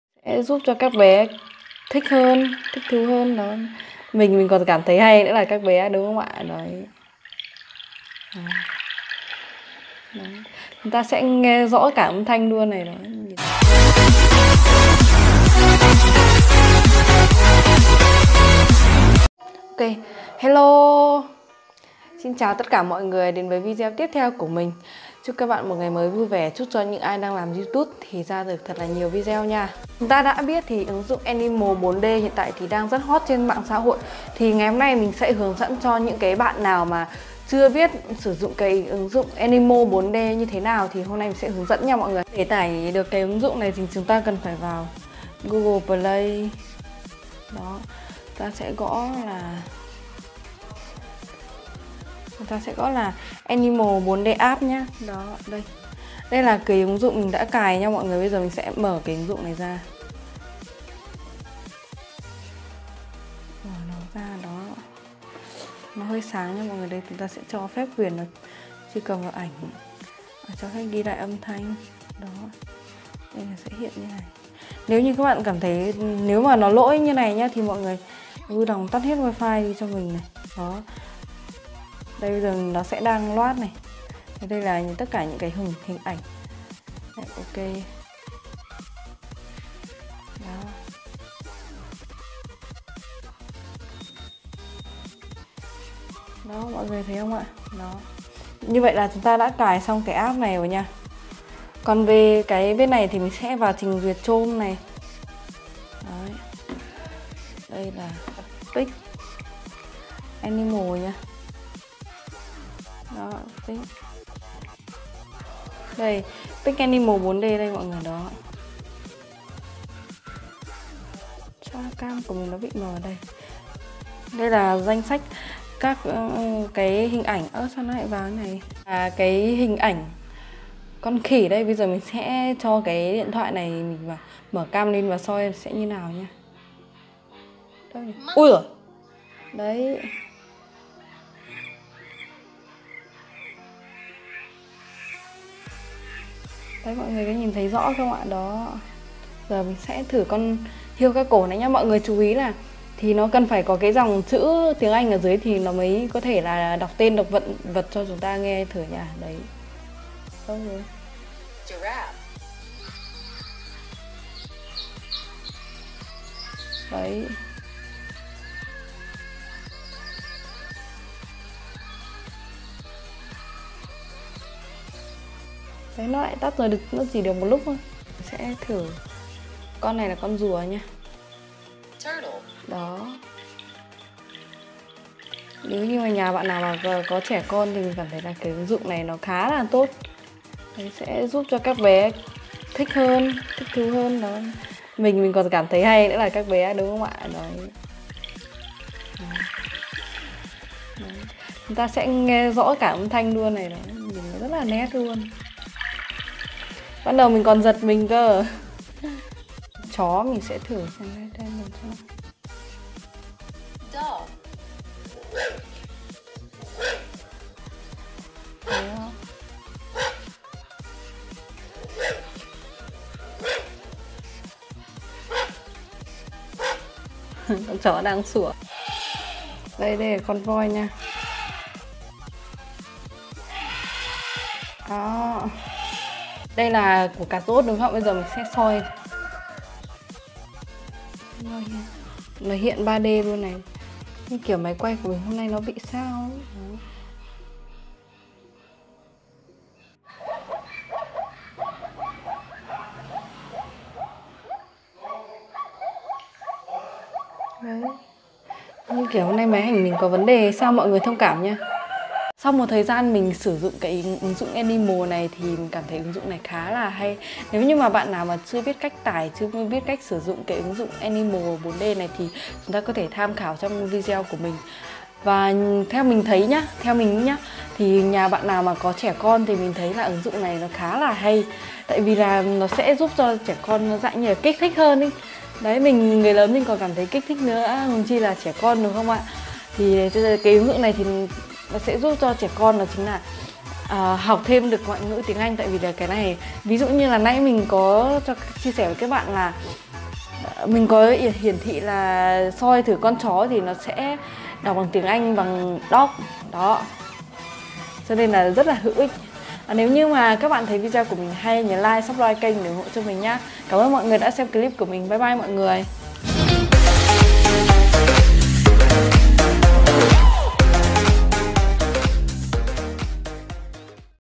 CÁCH TẢI VÀ SỬ DỤNG ỨNG DỤNG Animal 4d CHI TIẾT | Cói Lalin
Hướng dẫn tải ứng dụng Animal 4d chi tiết
Để tải ứng dụng này chúng ta cần phải có 2 chiếc điện thoại . Một chiếc tải ứng dụng animal 4d , một chiếc tải ảnh